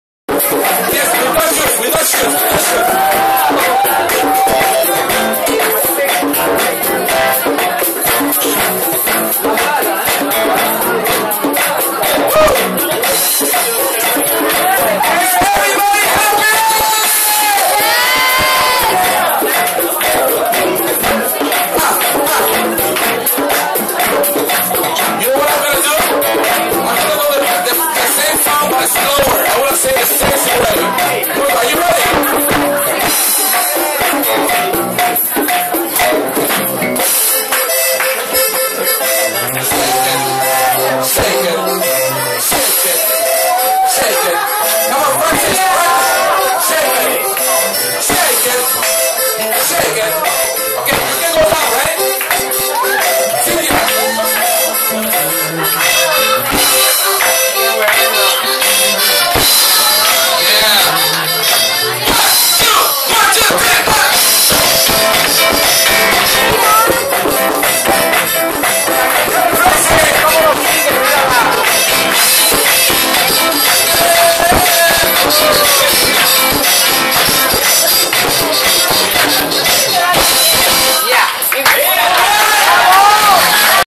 Latin Music Night @ El Cid Spanish Restaurant (Knutsford Terrace) 17 Oct 2012_Part 2
[Spanish National Day Celebration 2012]

Performance by Chris Polanco and Azucar Latina Band

Please LIKE our facebook page: El Cid Spanish Restaurant TST